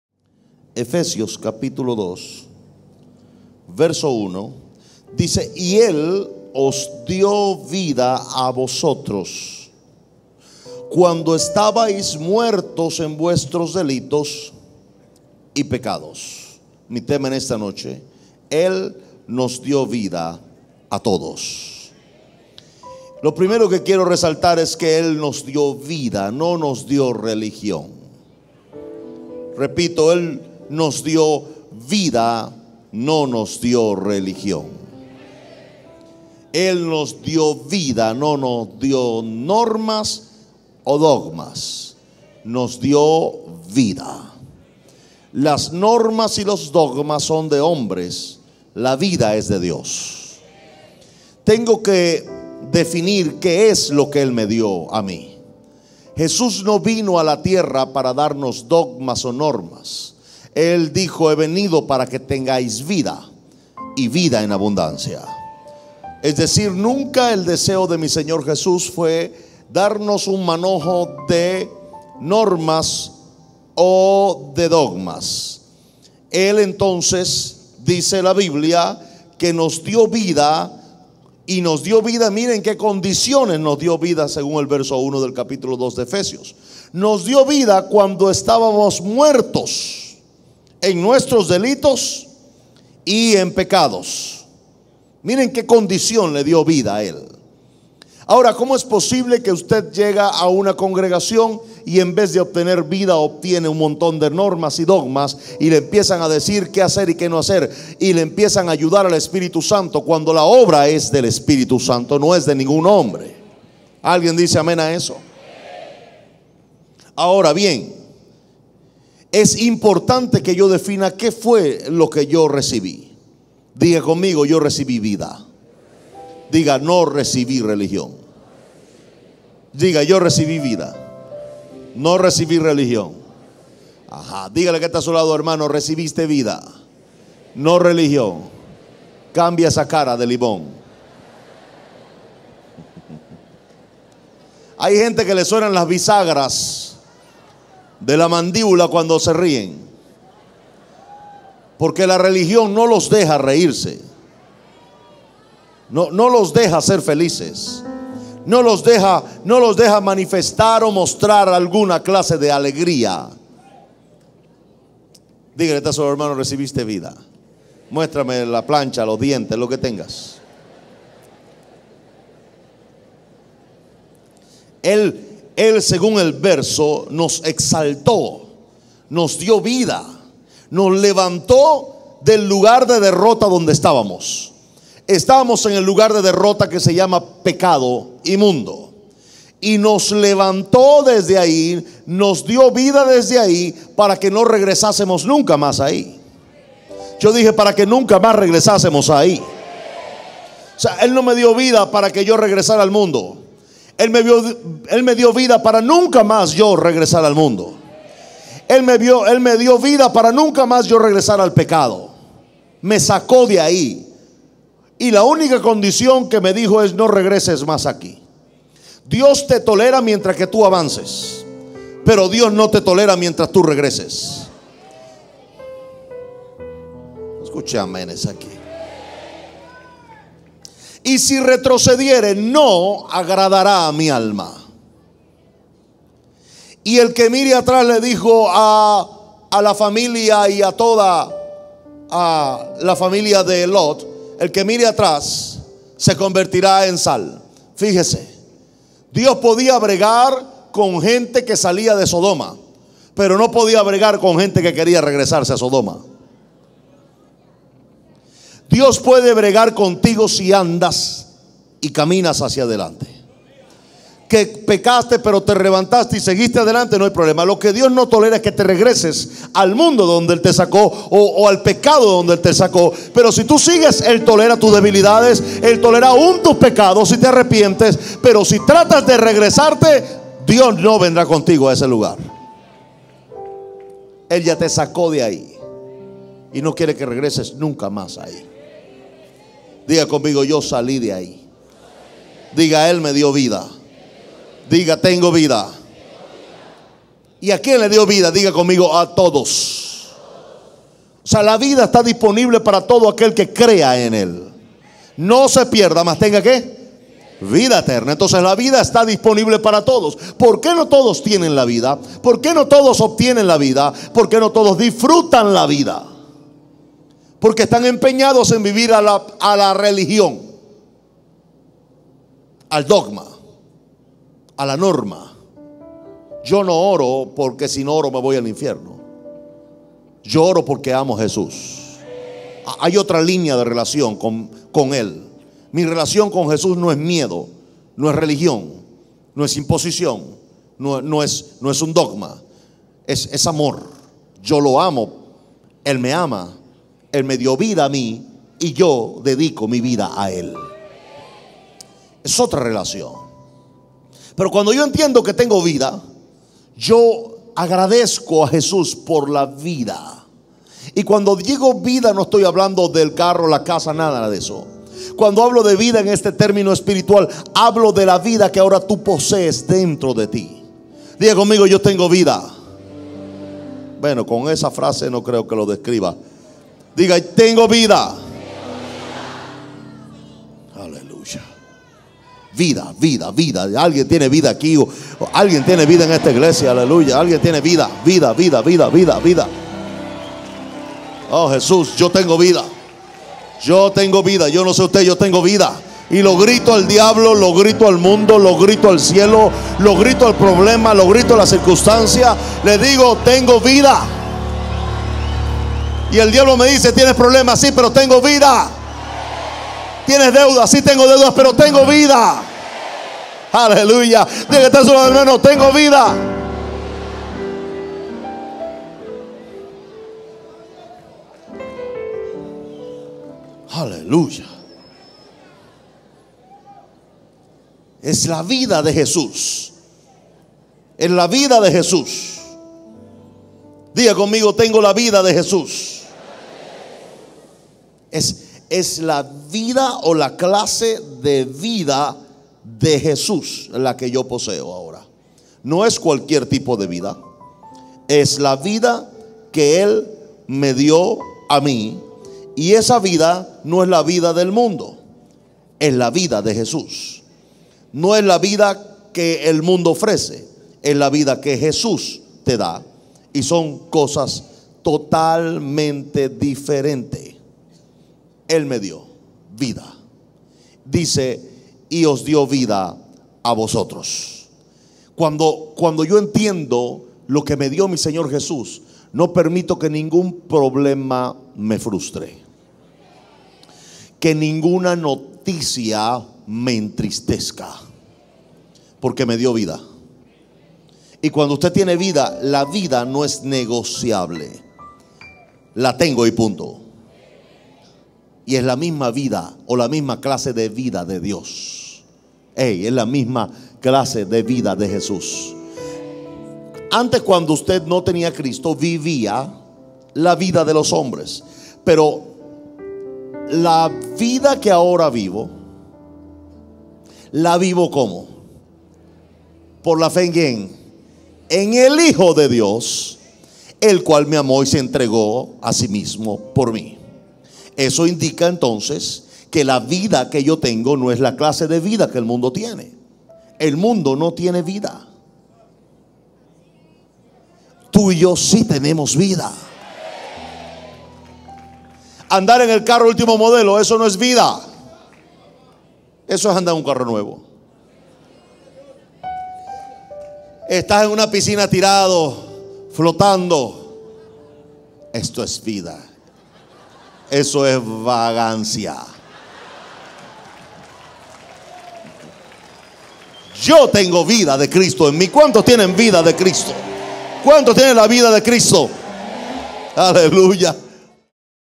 "Él nos dio vida a todos" Pastor Javier Bertucci (Viernes 31-05-2013)
La predicación: "Él nos dio vida a todos", dada por el Pastor Javier Bertucci´, el viernes 31 de mayo de 2013, comienza con la lectura de Efesios 2:1: "Y él os dio vida a vosotros, cuando estabais muertos en vuestros delitos y pecados". Porque Él nos dio vida, no "religión", no "normas" o "dogmas".  Los dogmas, la religión y  sus normas, son de hombres; en cambio, la "vida" es de Dios. Tenemos que definir lo que Jesús nos dio: Y vino para que tengamos vida y vida en abundancia.